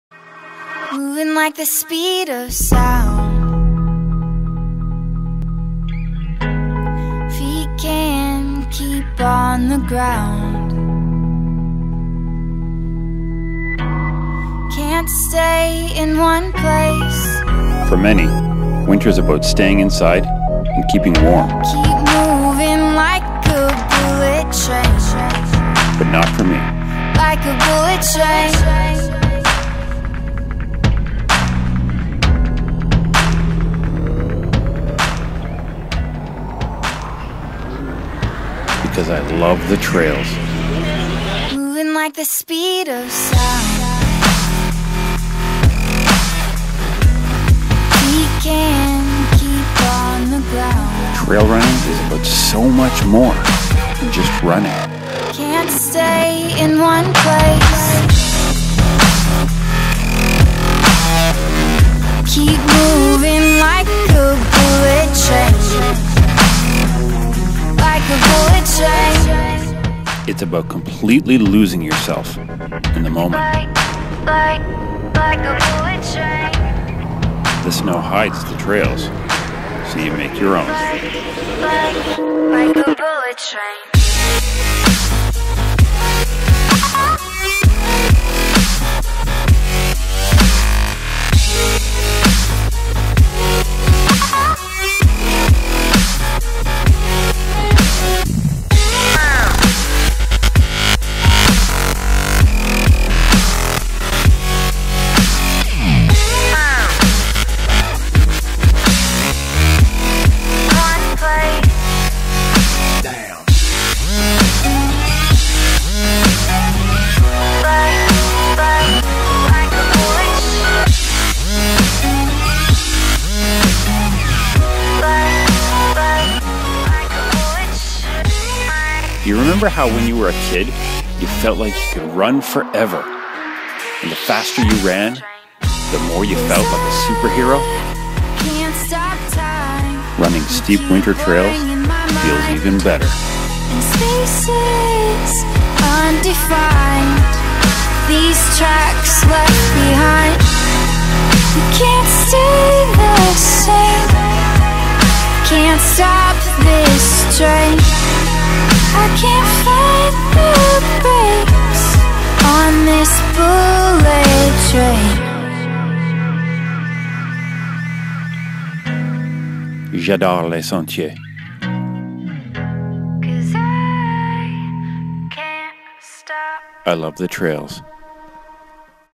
Trail Running in Winter - For the Love of Trails
The love of trail running doesn't stop when the snow flies.It just changes the scenery and makes things a bit more interesting.

There simply is no replacement for trail running. Road runners can use a treadmill during the cold winter months, but for trail runners, who have a true love of the trail, treadmill's, or dreadmills as I like to call them, offer no substitute. The only option is to continue running trails in the winter.